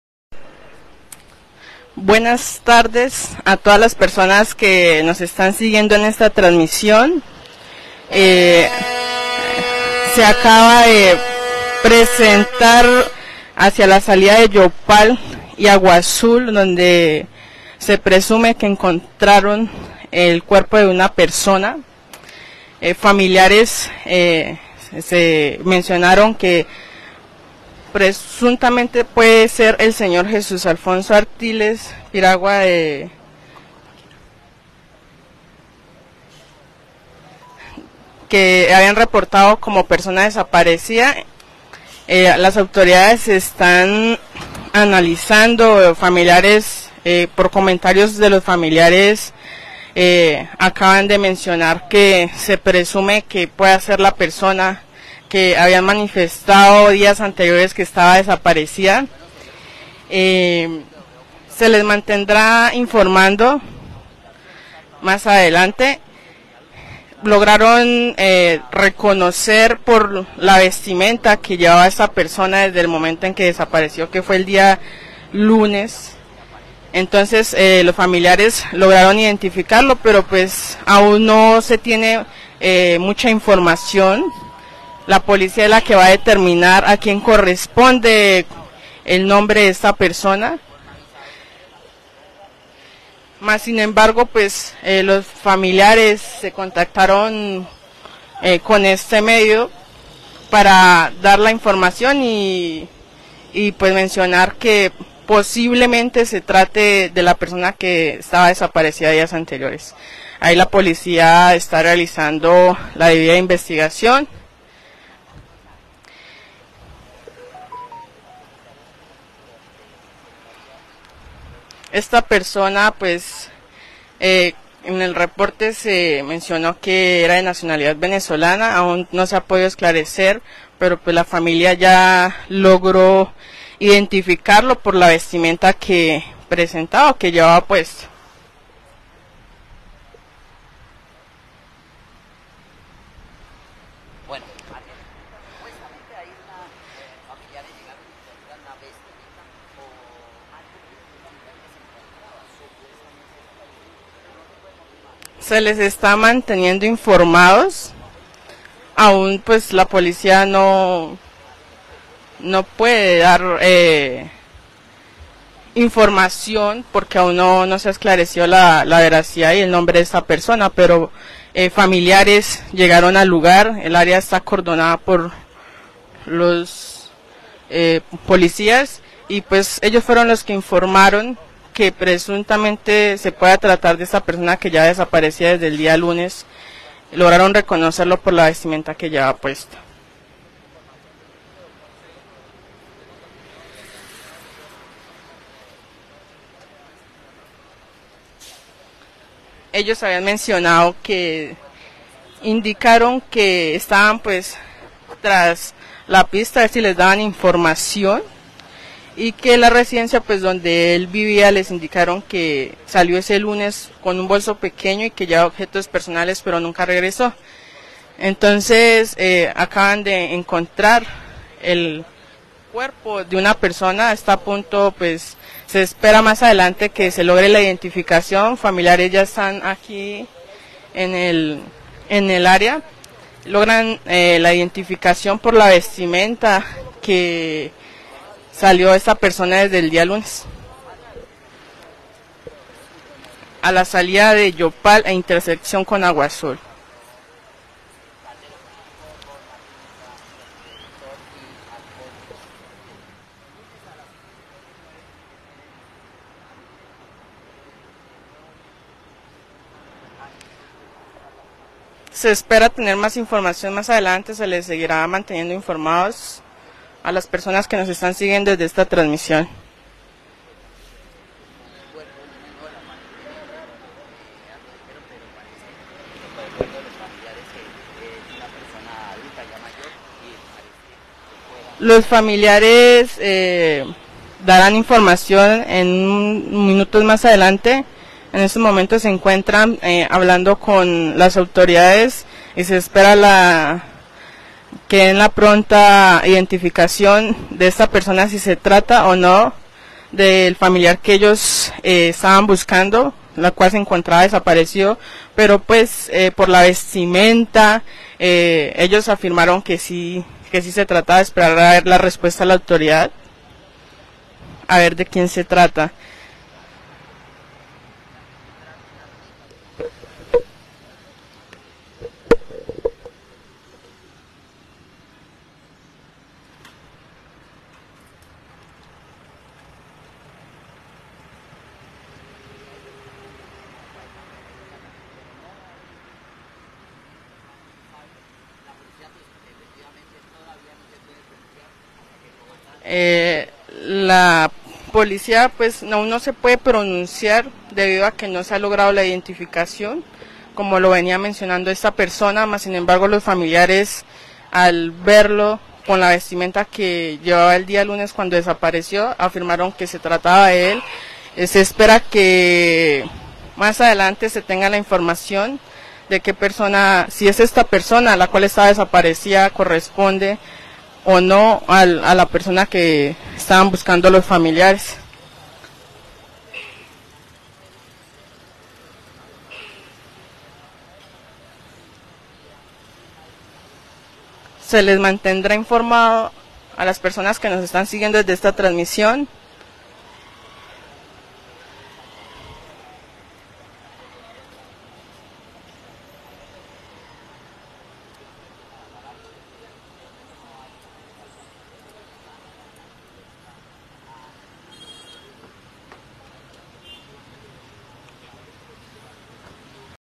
La policía inspeciona a un hombre fue hallado muerto en la vía Marginal del Llano a la salida Yopal
Un hombre fue hallado muerto en la vía Marginal del Llano a la salida de Yopal, se adelanta la inspección e identificación judicial a cadáver.